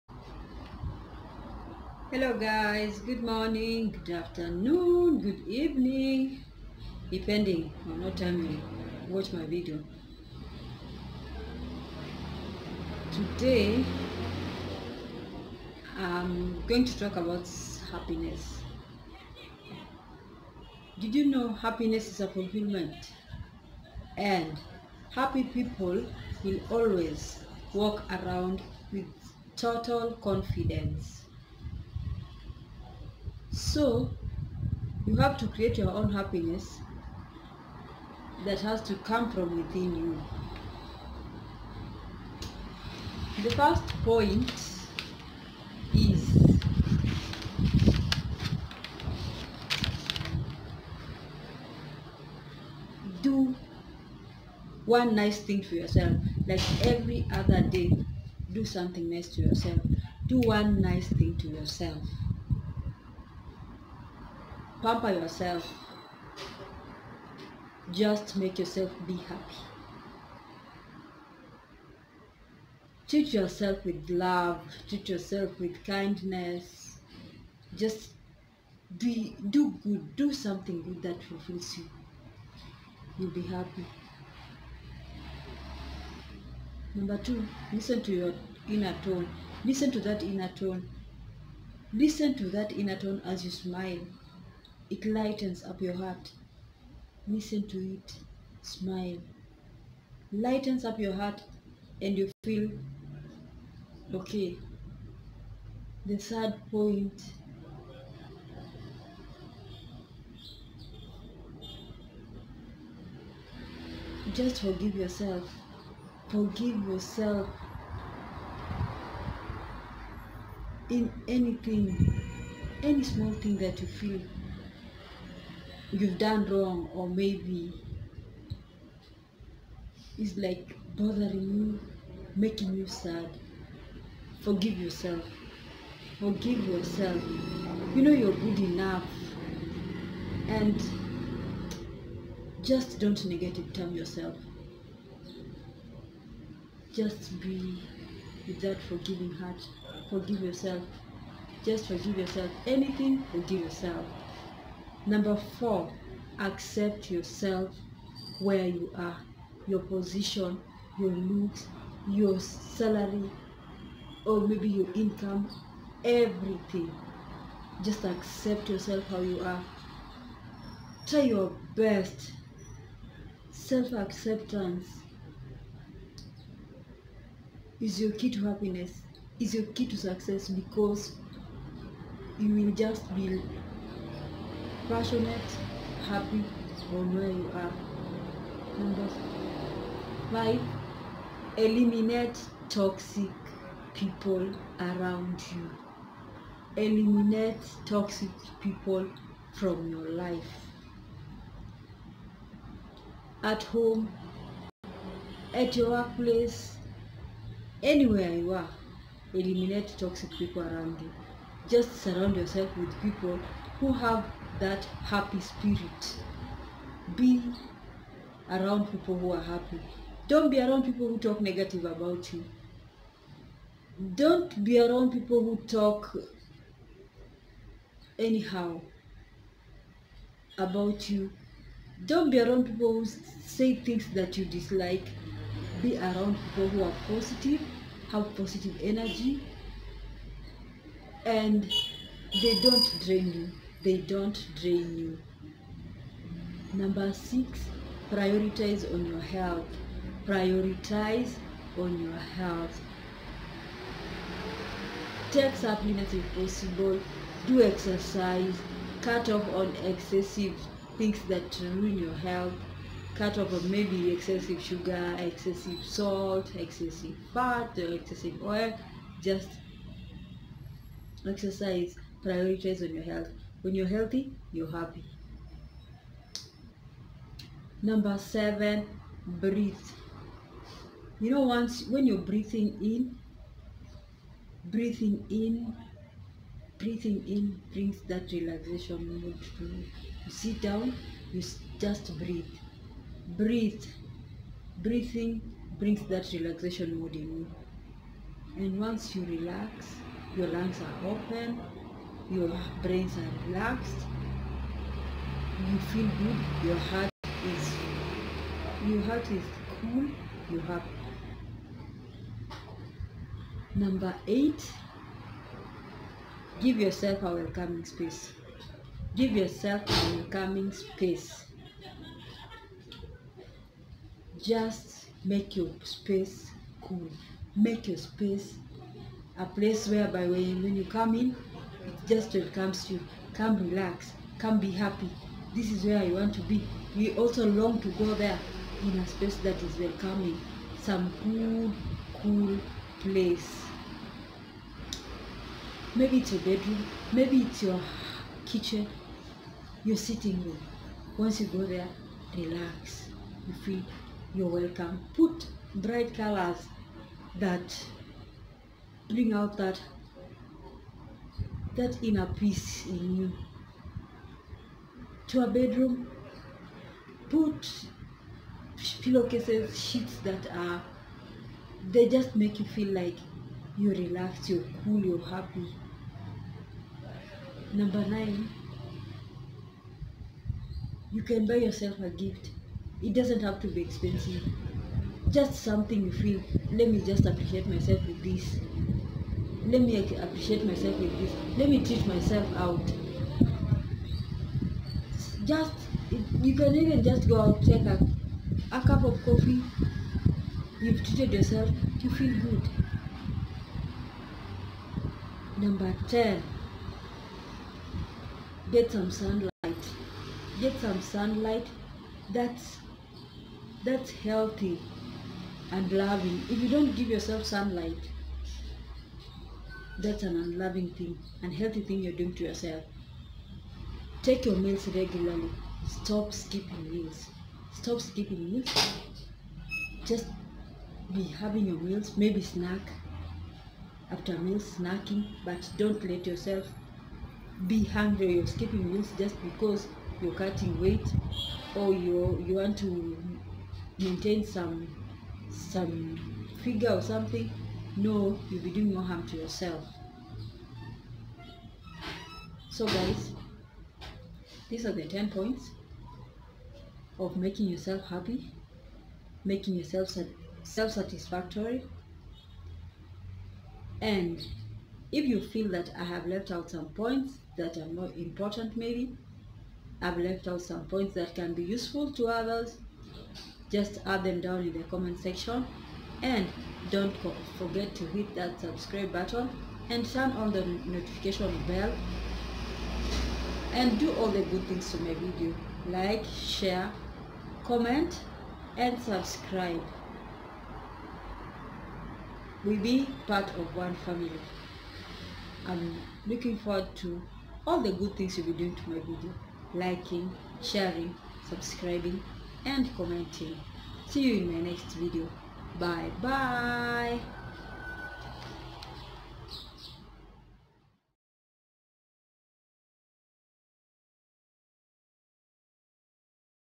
How To Be Happy Everyday: It will change your appearance
Know the difference between happy people and miserable people,its simple the glow on the face.